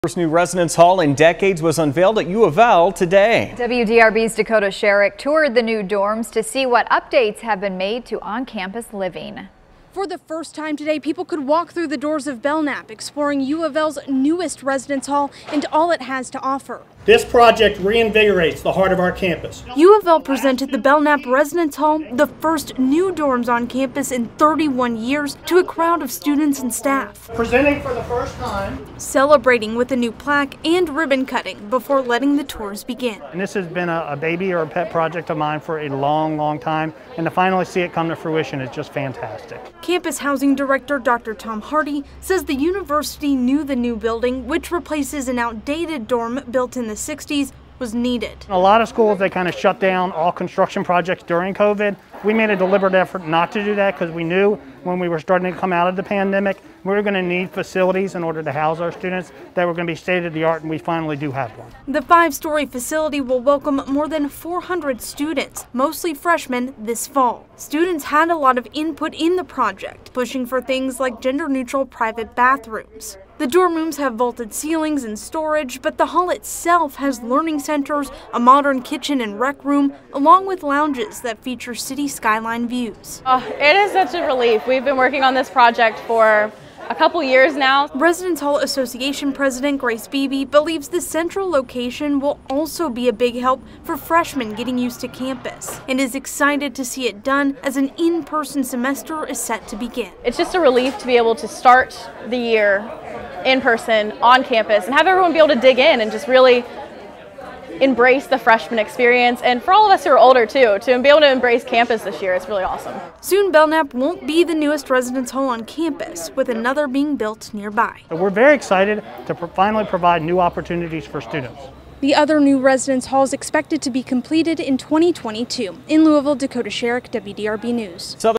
U of L dedicates new on-campus residence hall for first time in 31 years
The University of Louisville presented the Belknap Residence Hall to a crowd of students and staff Monday, the first new dorms on campus in 31 years.

The celebration came with a new plaque and ribbon-cutting before tours began.